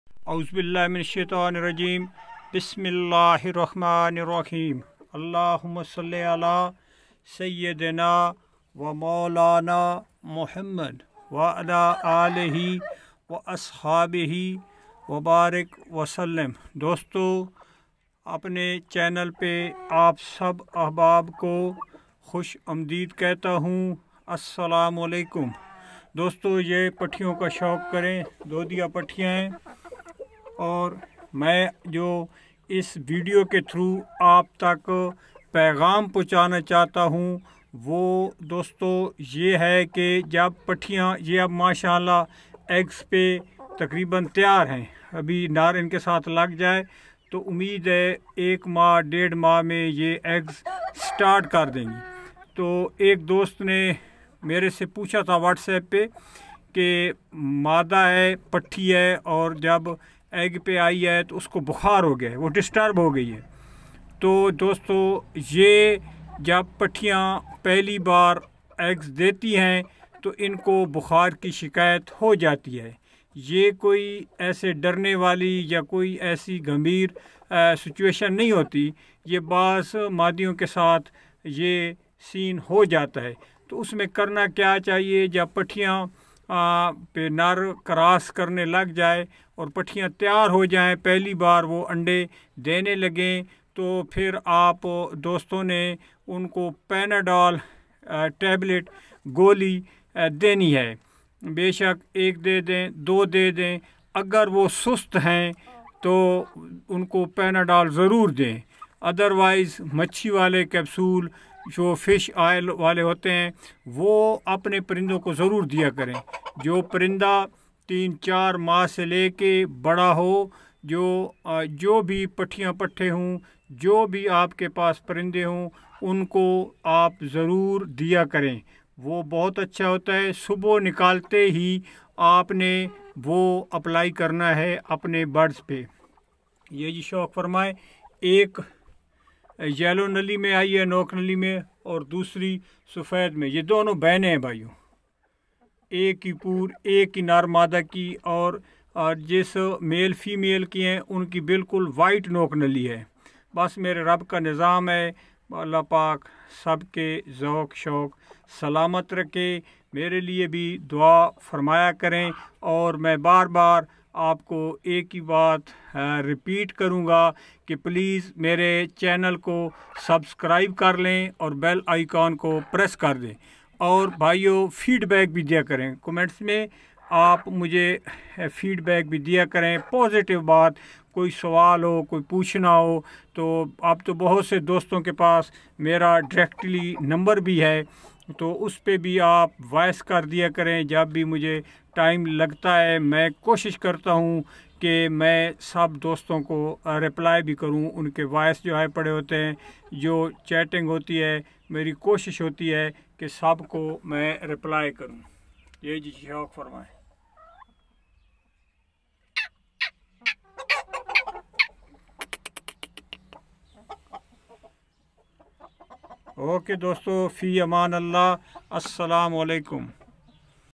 khalis breed|phli bar jo phtti eggs dae|agr bokhar ho jae|irfan bandial
Salam Friends|
I am here for you.I am fine and i hope
all of you fine.I love aseel and want to save this breed.
informative videos.
irfan bandial|
khalis breed|
breeding tips|